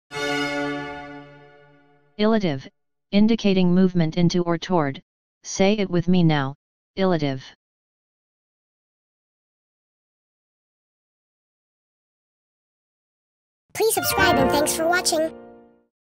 illative - Smart & Obscure English Words Defined 👁️🔊🗣🧠✅
This video is about "illative". Here's a video of some of the most obscure English words with definitions that will definitely make you sound smart (more smarter) if used properly.